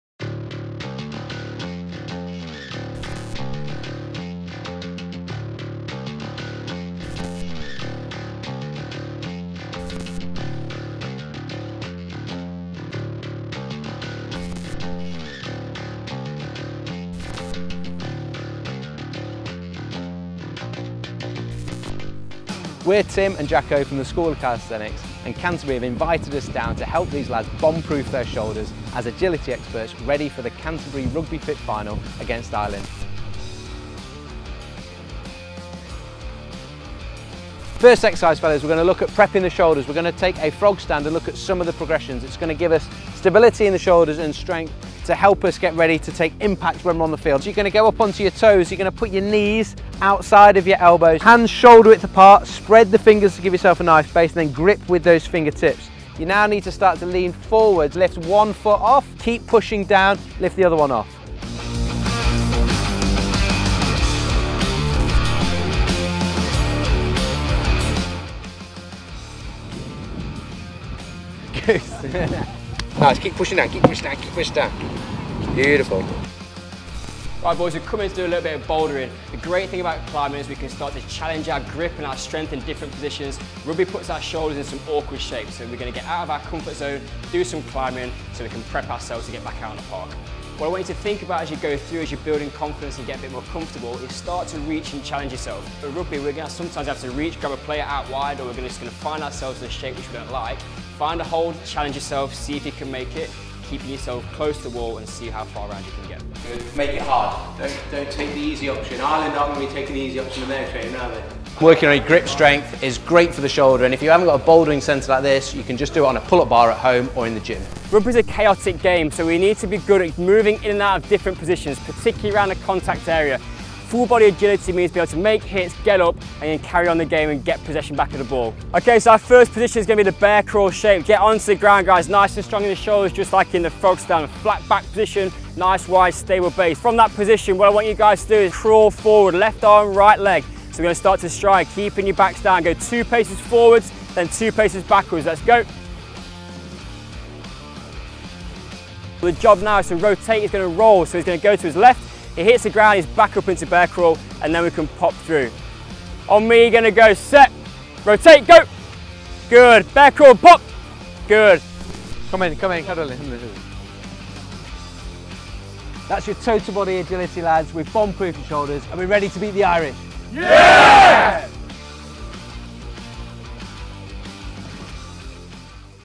Calisthenics for RUGBY | School of Calisthenics
We believe agility for rugby is more than just dazzling footwork and explosive change of direction. The best rugby players have total body agility and that means they can be a real physical presence all over the pitch, especially in the contact area.

When @canterburyNZ asked if we’d like to go back to our rugby roots and bring our innovate approach to agility to help Team England prepare for the RugbyFit final we jumped at the chance.

Calisthenics is an incredible way to bomb proof athletes shoulders for any impact so we were really excited to bring our training methods to the @rugbydump team in preparation for their game against Team Ireland.

The boys were awesome and they’re now equipped with total body agility skills to dominate the Irish at the RugbyFit final.

We wrote a complete training programme for the team and it's full of progressions so all the players can use it. We’ve also got more great news, after speaking to @canterburyNZ they have made it available to YOU for FREE too!

Head over to the Canterbury Hub for the FREE programme

There is some really fun stuff in the programme so we’d love to hear your feedback.